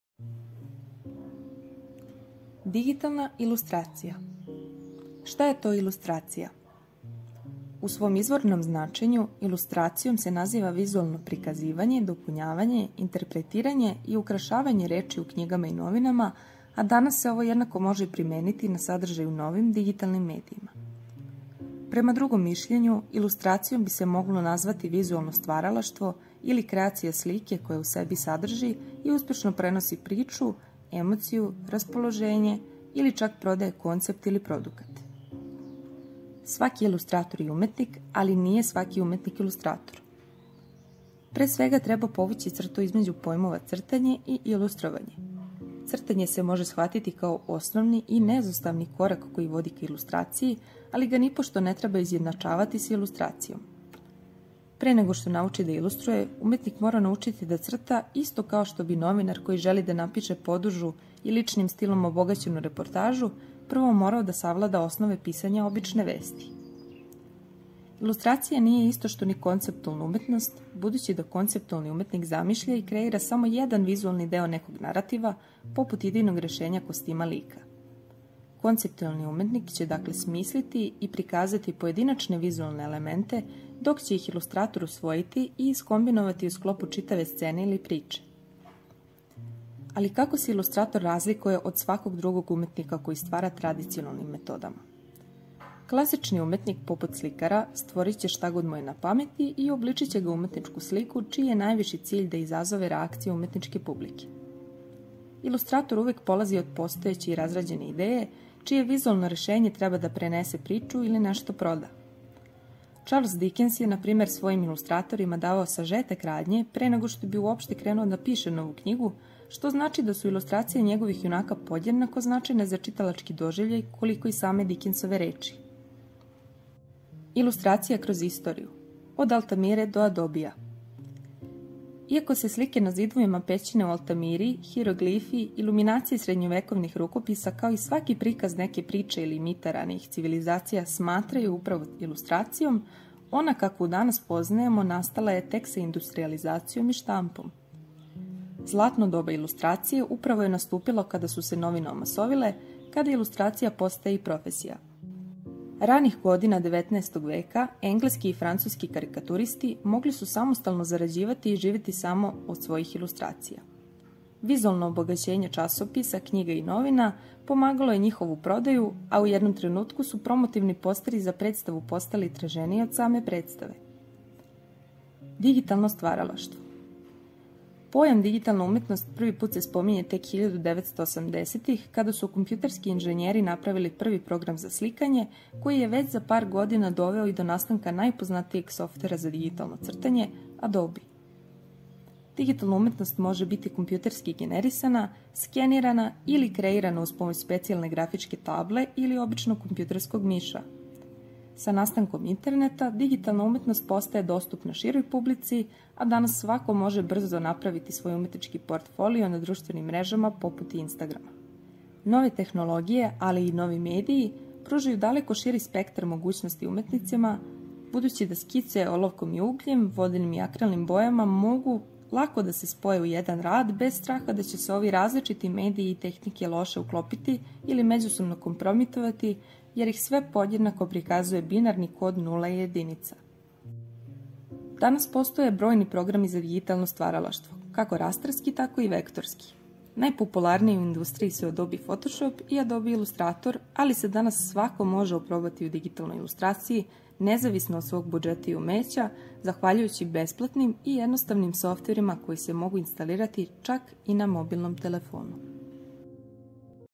ŠTA JE TO DIGITALNA ILUSTRACIJA?
Video u kojem objašnjavamo tehnike i fenomen digitalne ilustracije.

Autorke: Tamara Đokić, Emilija Kostić